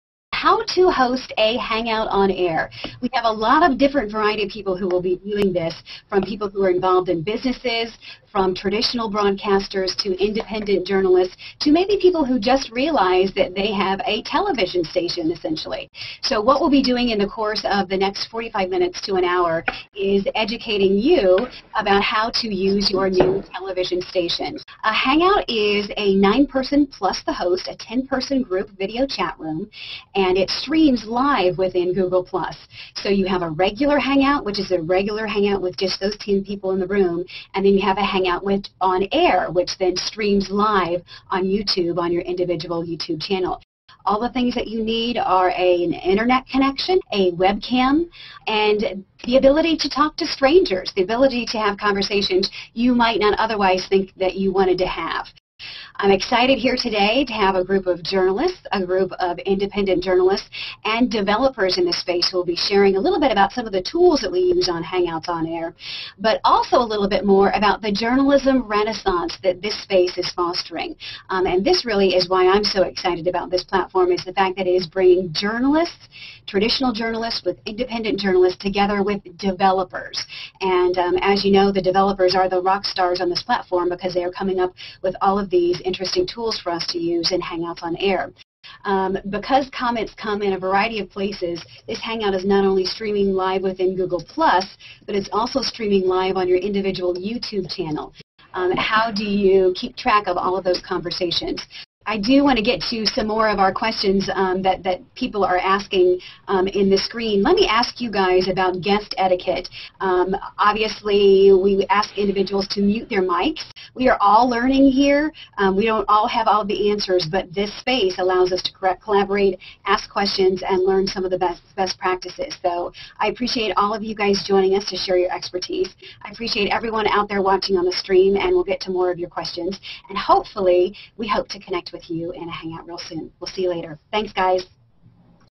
How to Host a Hangout On Air with Sarah Hill - Super 8 version
On 3 July 2012, Sarah Hill and guest showed us how to Hangout On Air. Here's my edit as a Super 8 Limited Edition.  The original has over 9,000 views, find the full 45 minute broadcast
This edit was created using offline Sony Vegas and the YouTube Video Editor